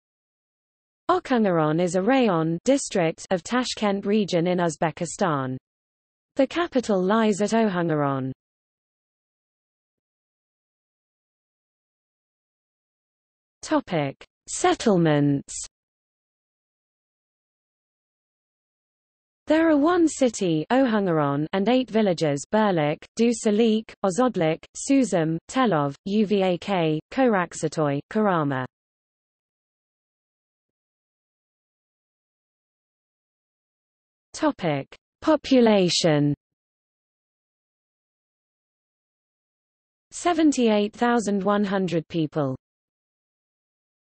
Okhangaron District | Wikipedia audio article
00:00:10 Settlements
00:00:30 Population

- Socrates

SUMMARY
Okhangaron is a raion (district) of Tashkent Region in Uzbekistan. The capital lies at Ohangaron.